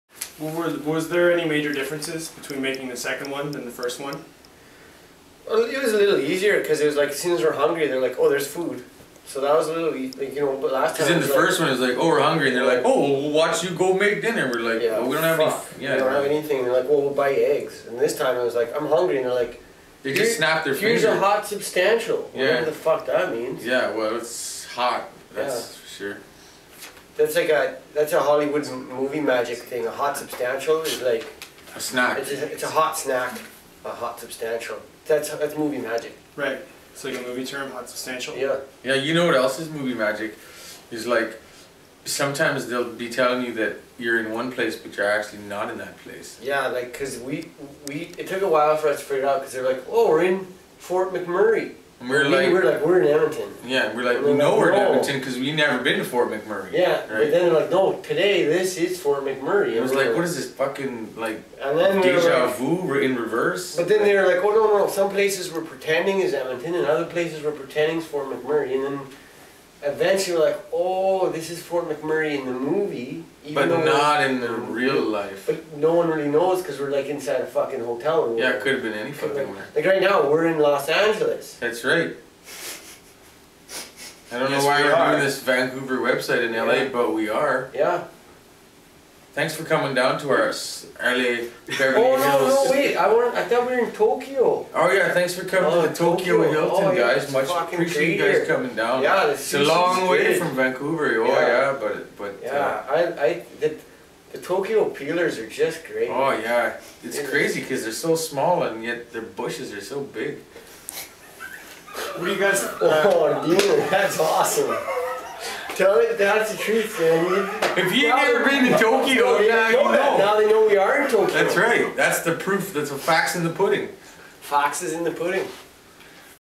Terry and Deaner on Movie Magic and Tokyo Strippers
Terry and Dean from Fubar 2 teach ION Magazine about movie magic and Tokyo Strippers.